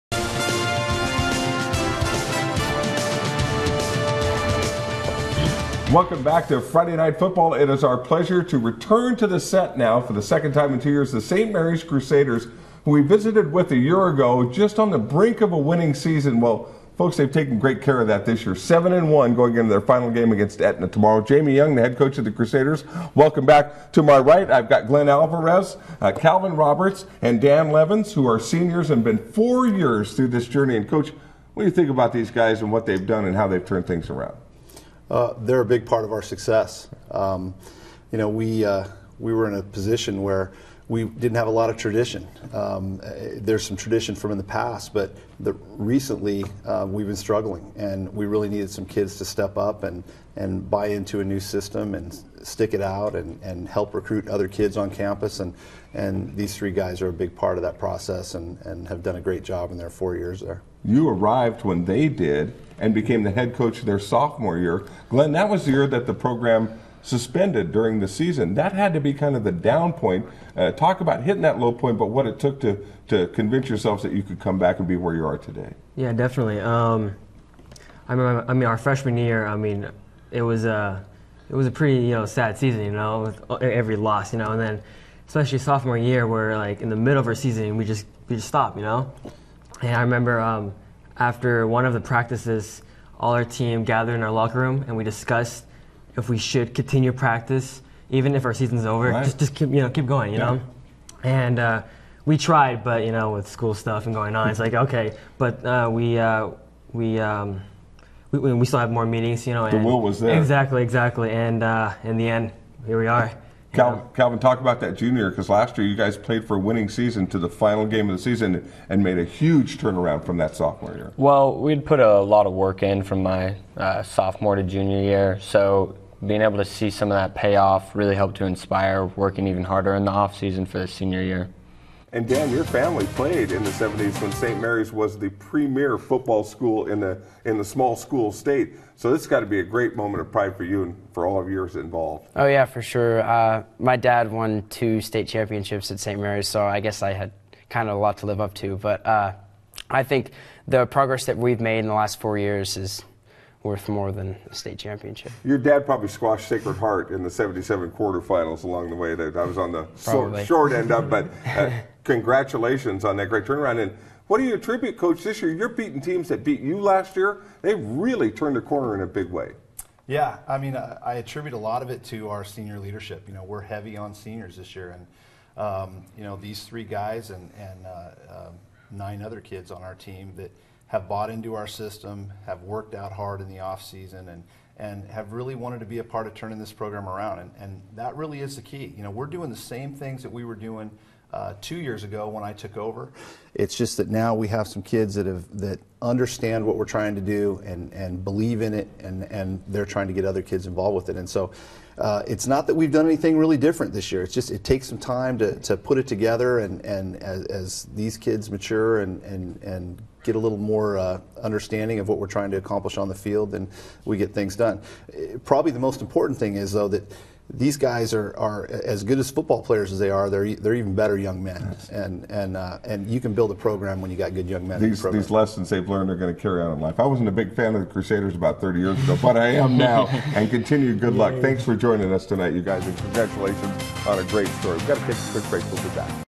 Seniors have seen St. Mary
Crusader seniors know where they have been and how far they have come.The St. Mary players and coach joined News10's Joe Brett to talk about how their program has grown over the last four years.The Crusaders will play their last regular season game against Etna on Saturday. This season, St. Mary's has lost only one game.